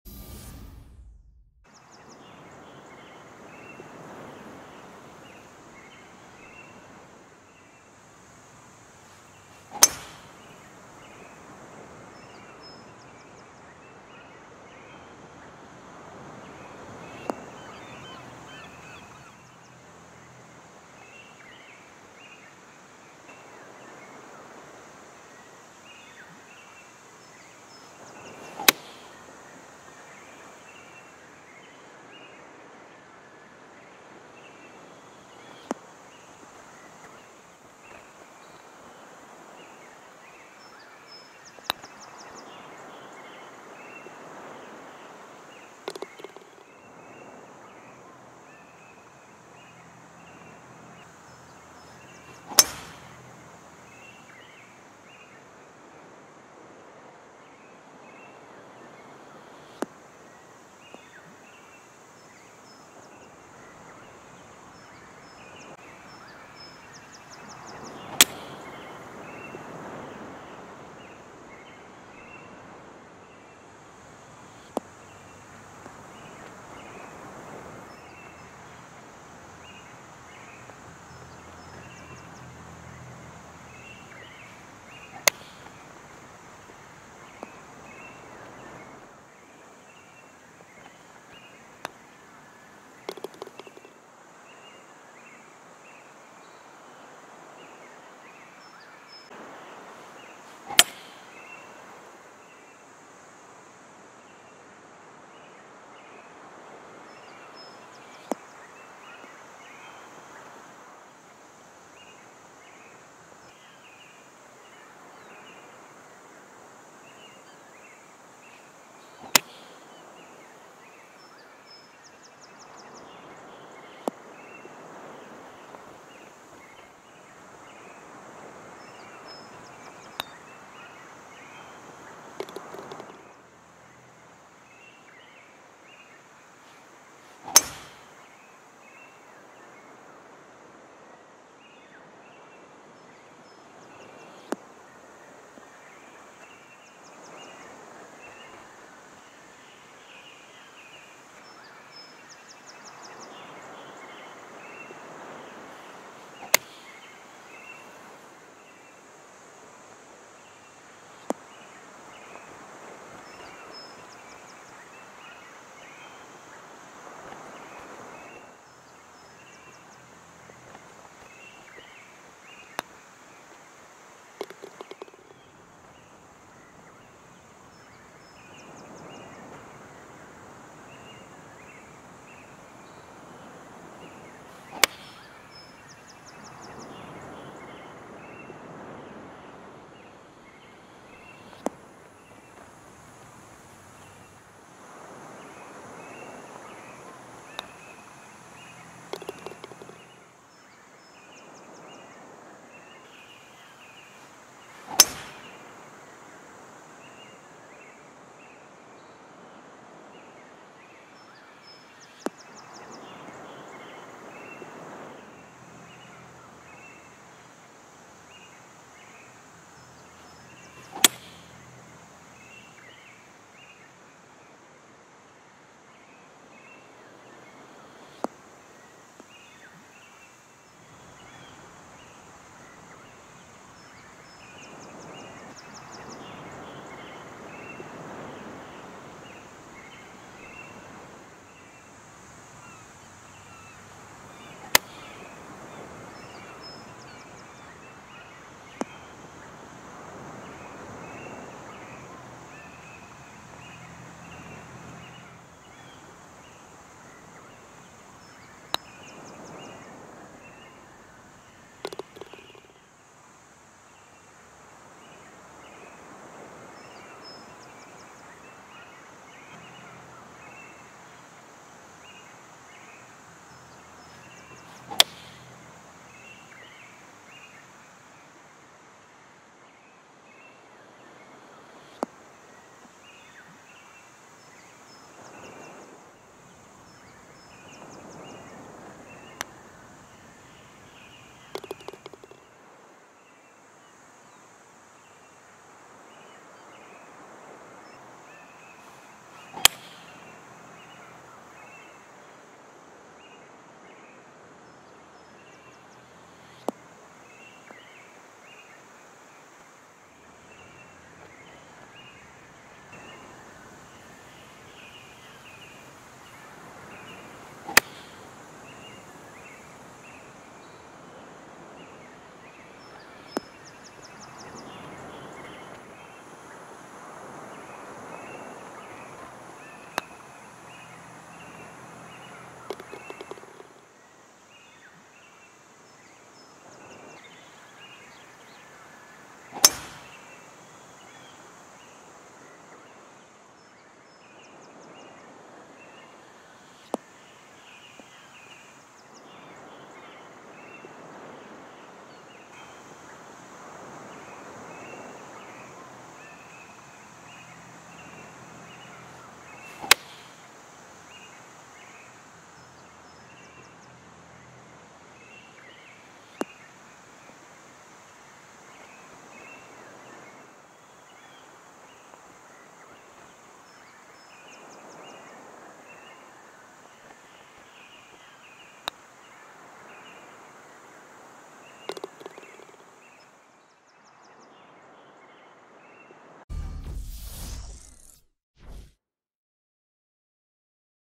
Chelsea Saint @ Tantallon Castle Front 9 | 'As LIVE' Edit
Precisely what it says on the tin.

Hey, I enjoyed Sneak Peek 114 so much - I went straight back out there and played a rare 9 holes from the back. And here's the 'As Live' 6½ minute edit of that cool little visit. Fun course - fun watch..